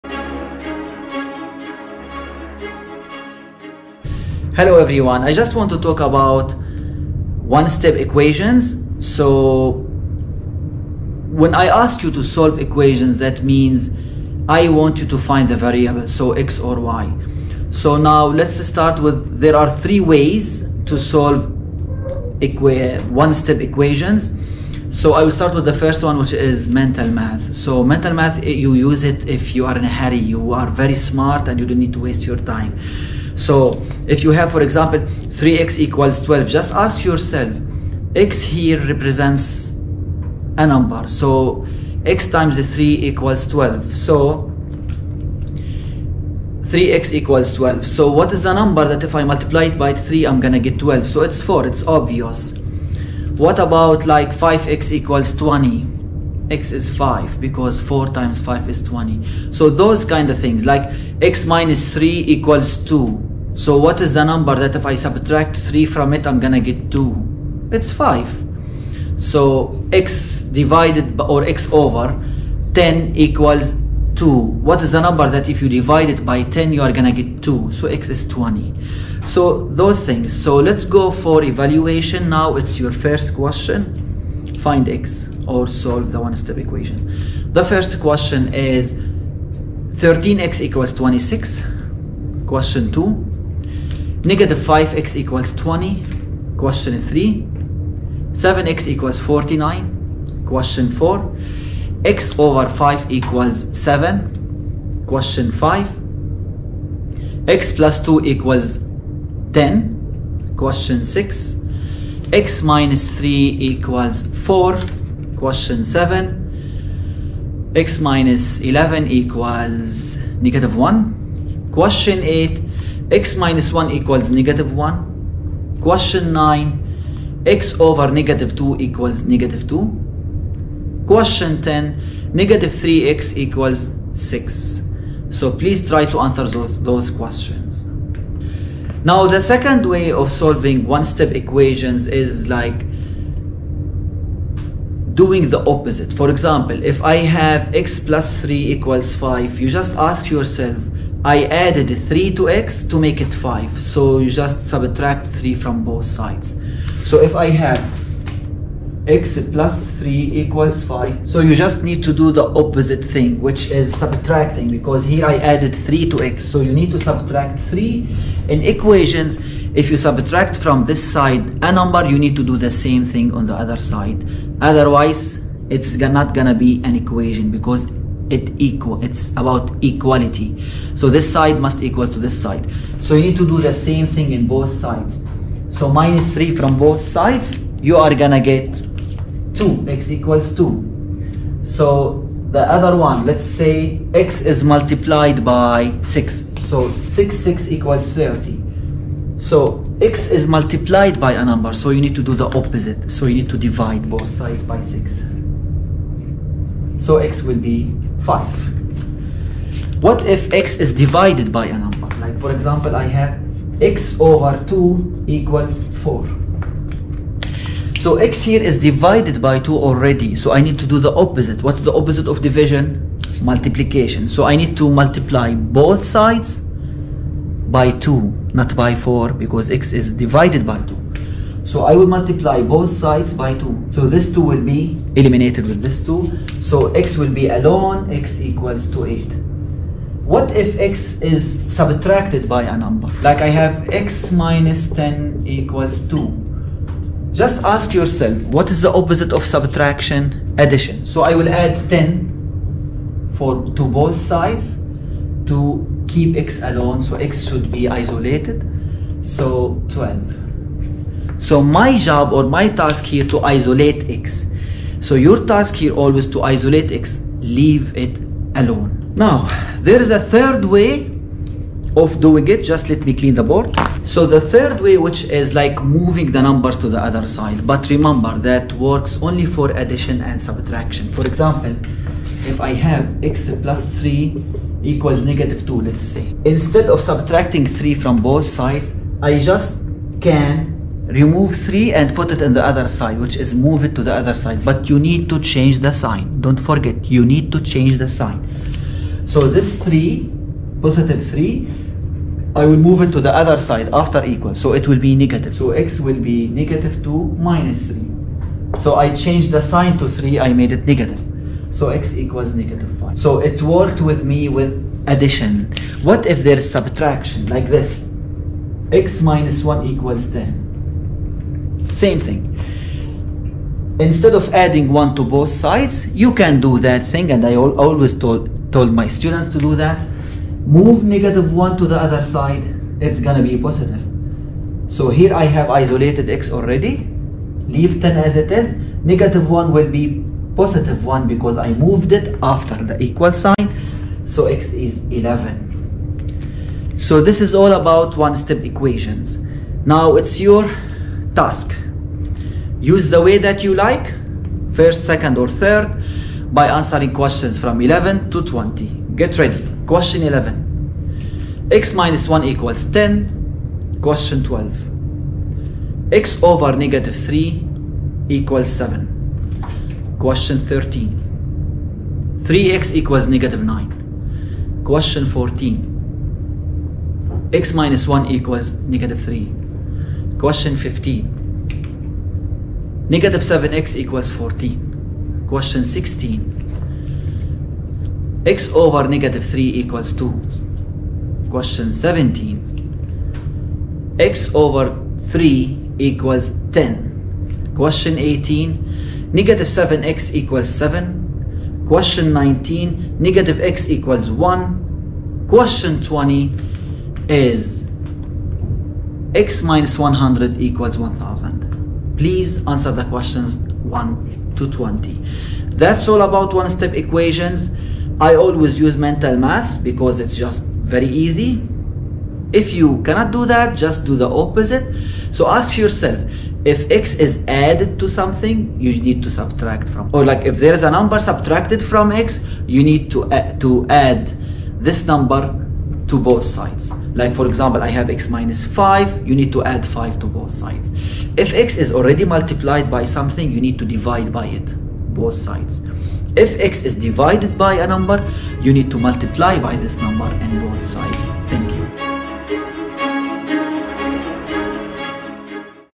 Solving One-Step Equations
An easy way to solve one-step equations and students have to answer the questions while watching the video.